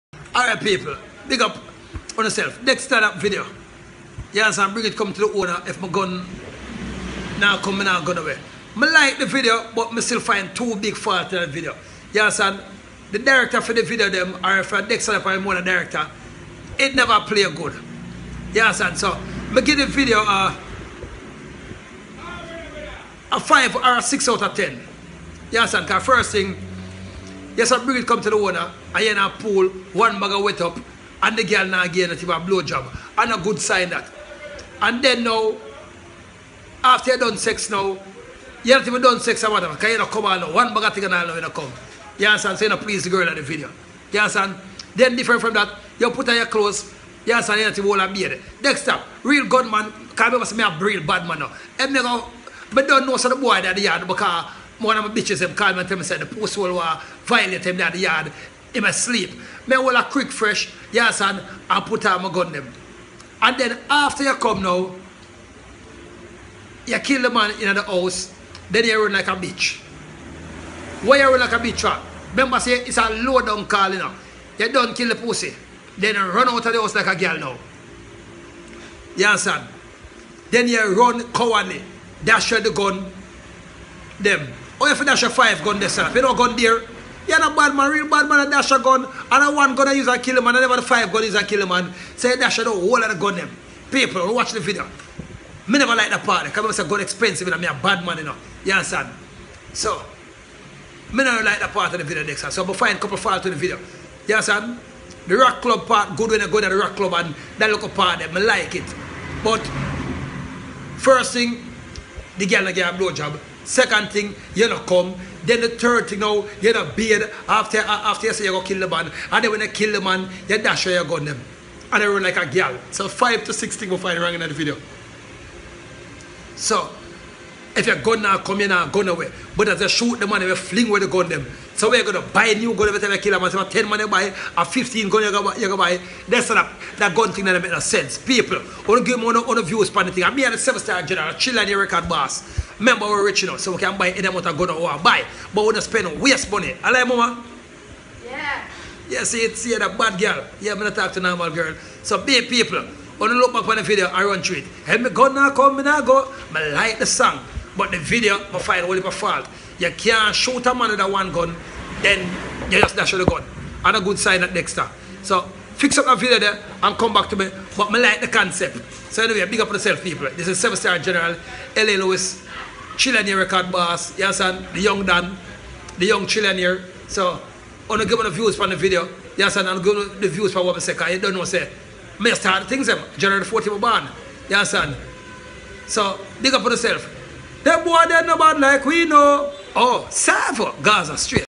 LA Lewis Calls Dexta Daps New Music Video Fuckry
LA Lewis Says Dexta Daps New Music Video is "FUCKRY!!!"

Office: 1C Grants Crescent, Kingston 10. Jamaica.